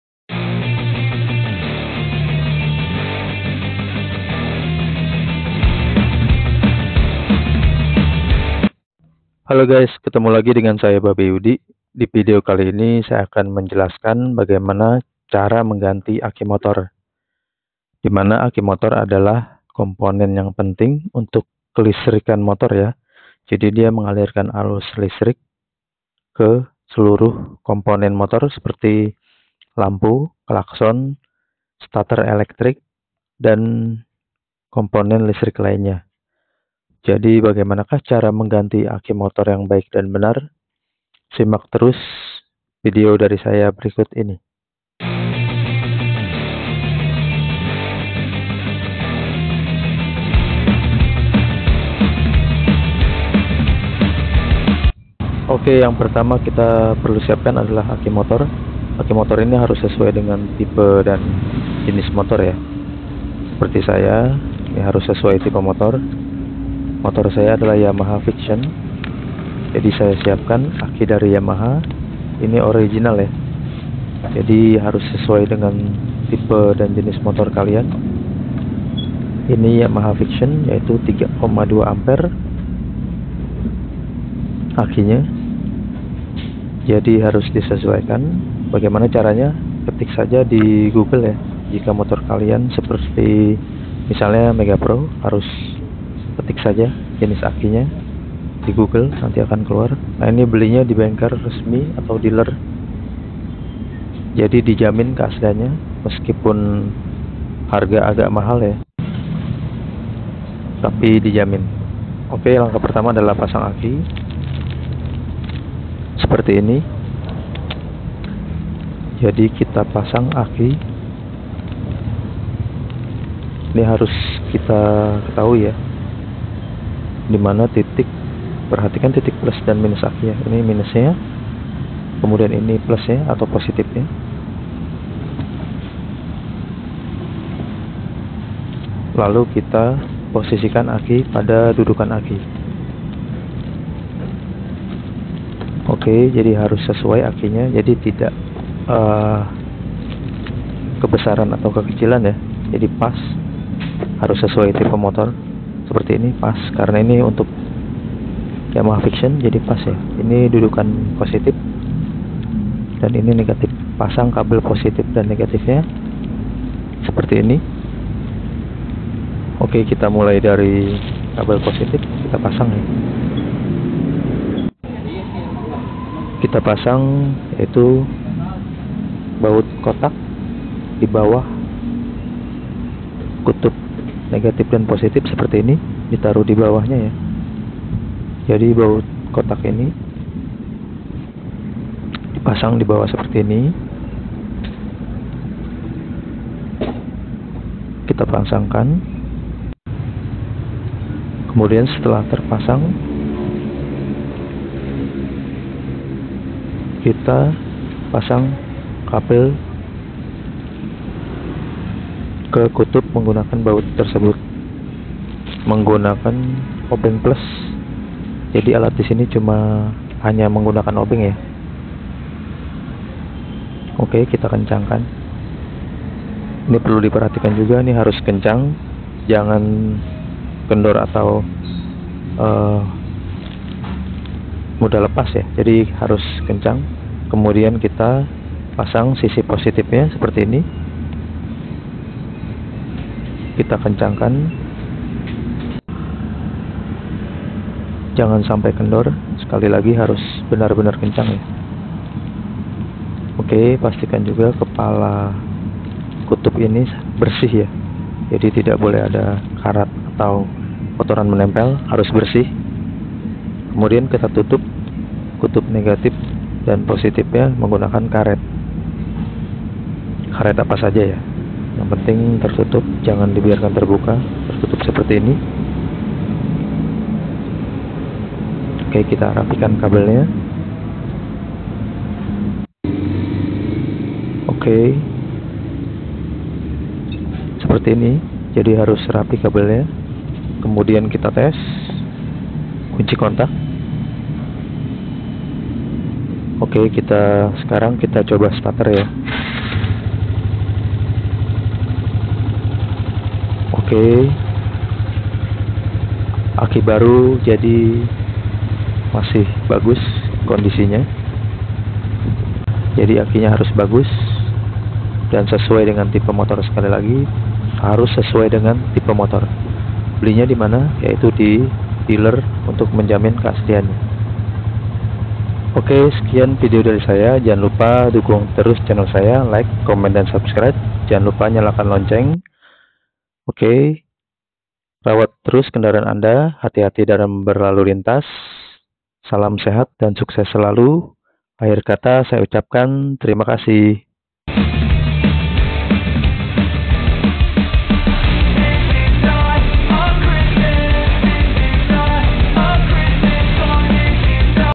Cara mengganti aki motor | pasang aki yang benar
Berikut adalah Video Cara mengganti aki motor dengan baik dan benar.